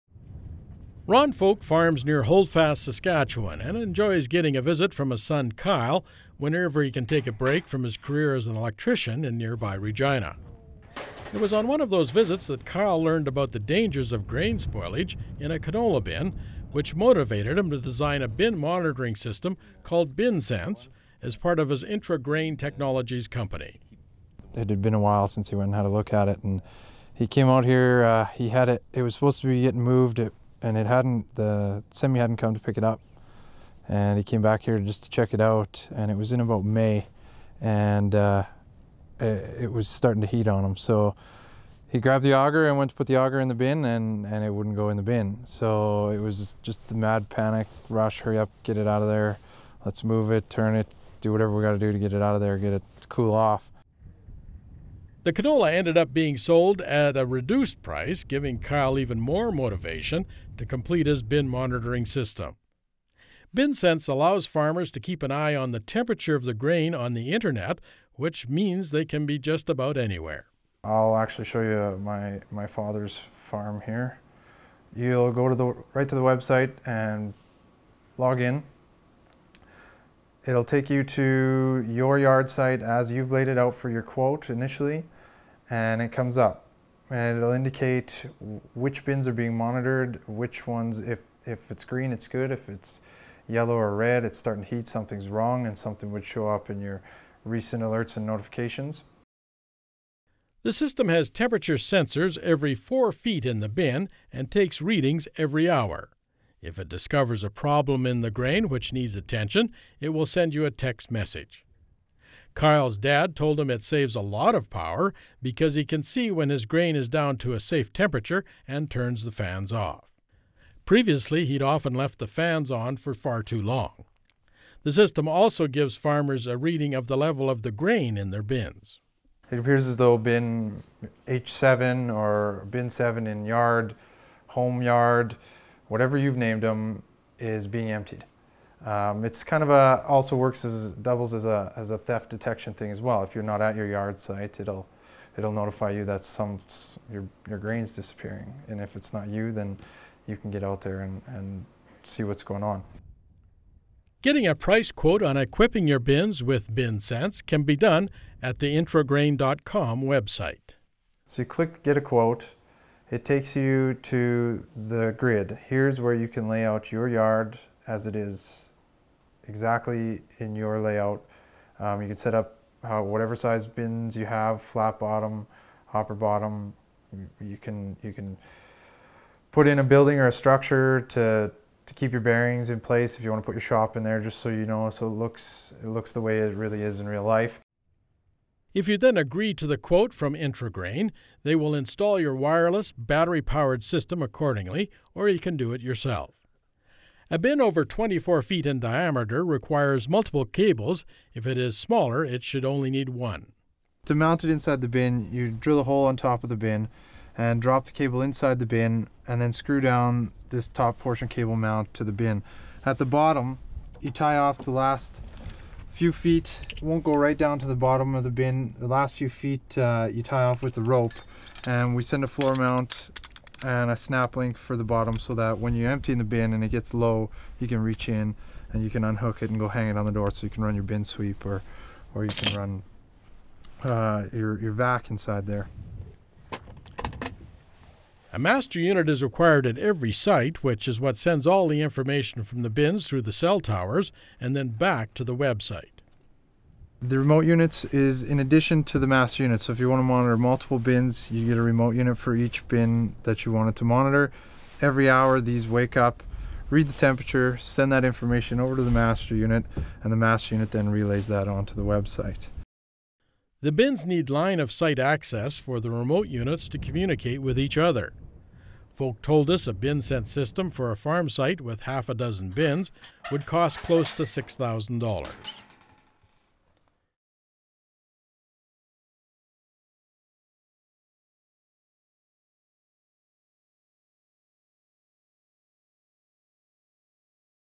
Wireless Grain Bin Monitoring
Features: Kyle Folk owner of IntraGrain Technologies Inc., has seen first hand the dangers of grain spoilage in a canola bin having been raised on a farm near Holdfast, Sk. As a result he got to work and developed a bin monitoring system that can be accessed online so farmers can keep a close eye on the temperature of grain stored in bins. At the time of our visit in 2012 the cost for a monitoring system for a half a dozen grain bins was about $6,000.00.